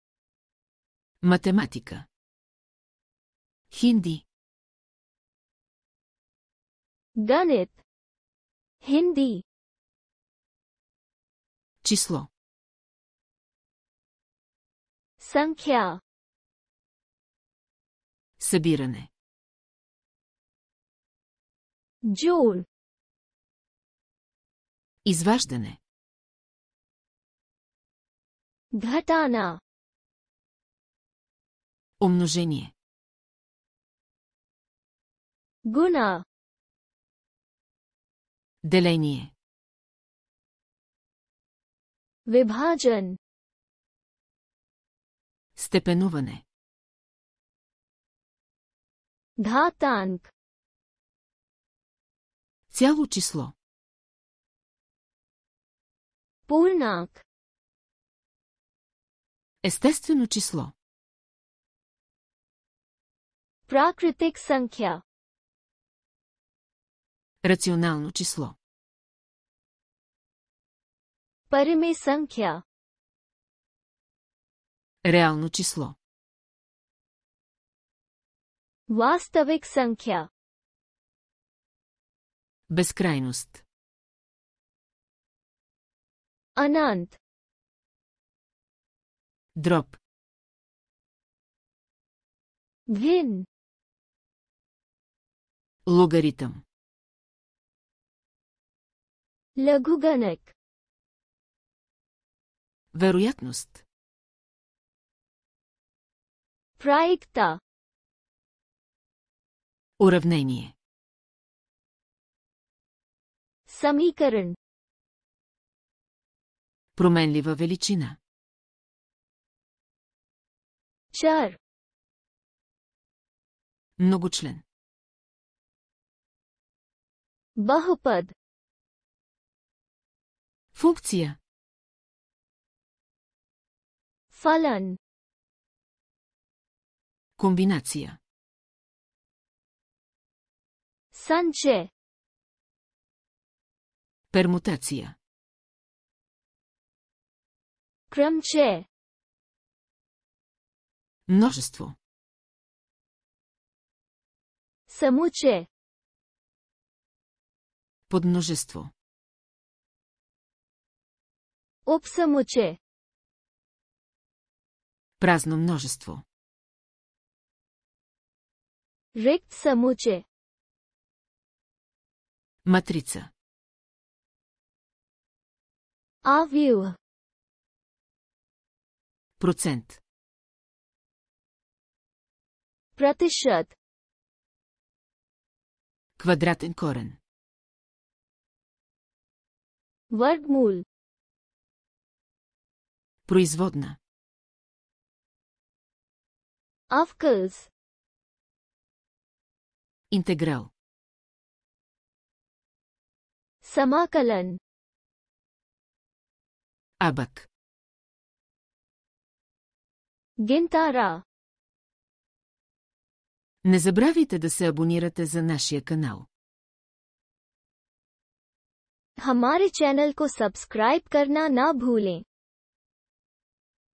Математика (Хинди) (bg-hi)
Fone TV
bg-hi-35
गणित (हिन्दी)
Число - संख्या
Събиране - जोड़
Изваждане - घटाना
Умножение - गुणा
Деление - विभाजन
Степенуване - घातांक
Цяло число - पूर्णांक
Естествено число - प्राकृतिक संख्या
Рационално число - परिमेय संख्या
Реално число - वास्तविक संख्या
Безкрайност - अनंत
Дроб - भिन्न
Логаритъм - लघुगणक
Вероятност - प्रायिकता
Уравнение - समीकरण
Променлива величина - चर
Многочлен - बहुपद
Функция - फलन
Комбинация - संचय
Пермутация - क्रमचय
Множество - समुच्चय
Подмножество - उपसमुच्चय
Празно множество - रिक्त समुच्चय
Матрица - आव्यूह
Процент - प्रतिशत
Квадратен корен - वर्गमूल
Производна - अवकलज
Интеграл - समाकलन
Абак - गिनतारा